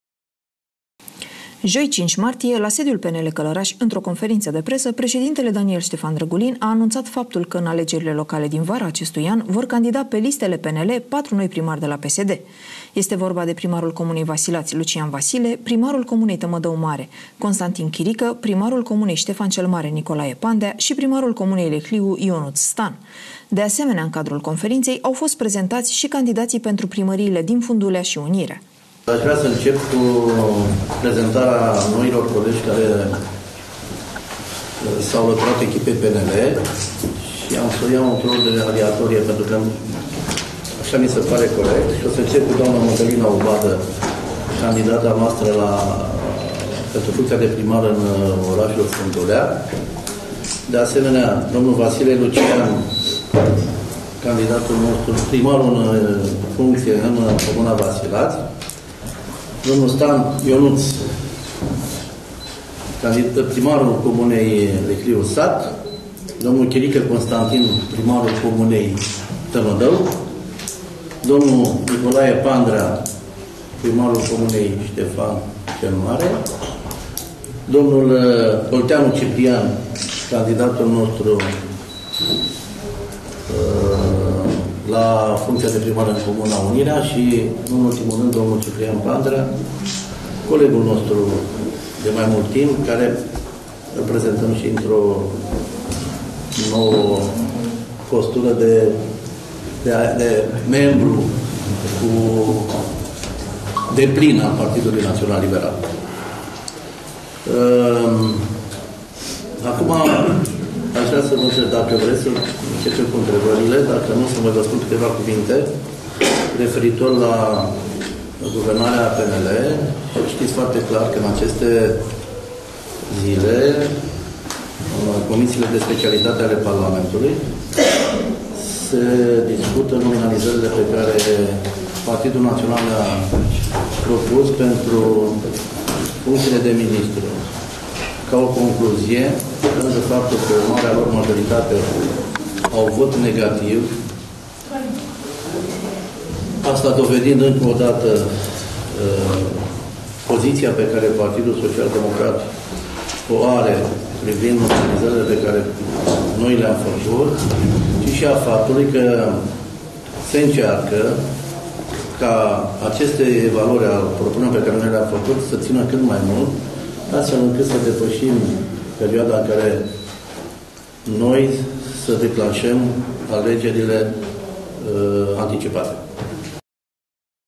Știri Locale - Patru primari PSD au trecut la PNL
Este vorba de primarul comunei Vasilați, Lucian Vasile, primarul comunei Tămădău Mare, Constantin Chirică, primarul comunei Ștefan cel Mare, Nicolae Pandea, și primarul comunei Lehliu, Ionuț Stan.